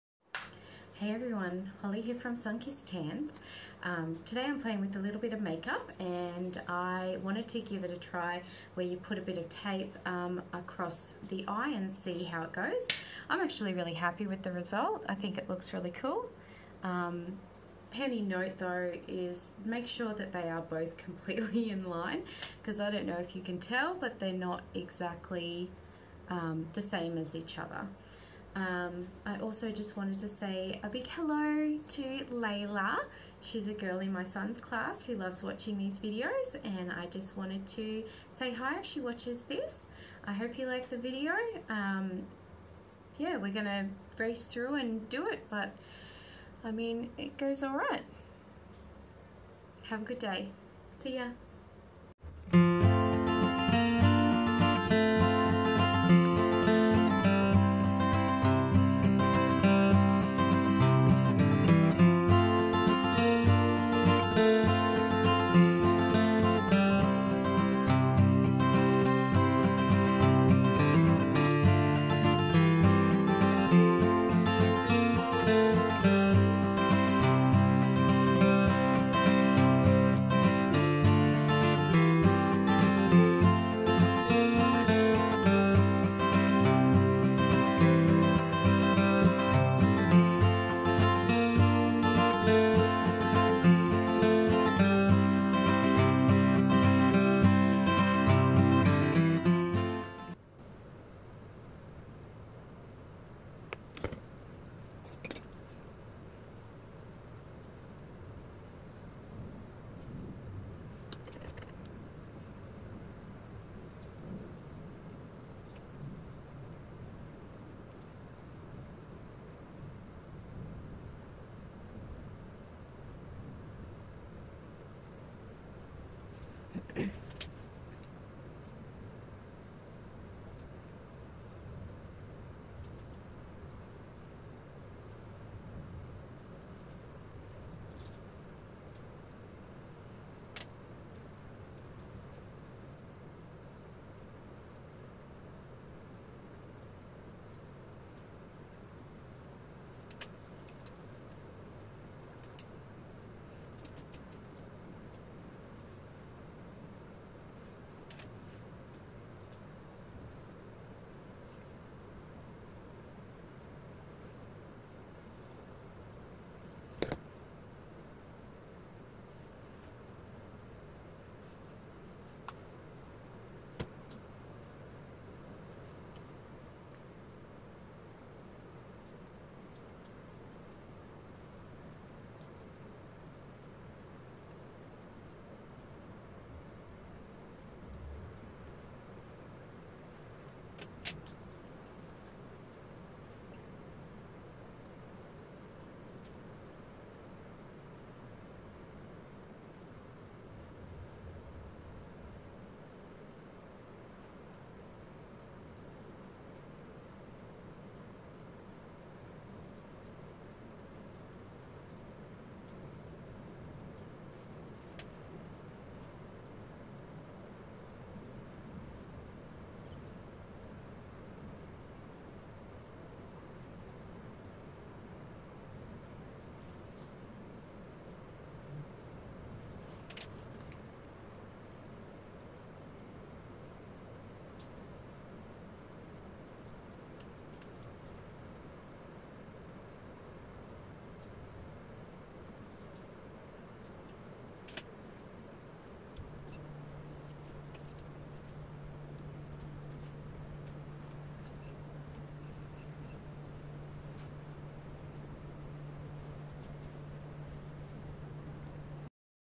Makeup Tutorial with tape
We wanted to try out the "tape trick" for ourselves. Pretty happy with the result! We use only the best - Kylie's Professional mineral makeup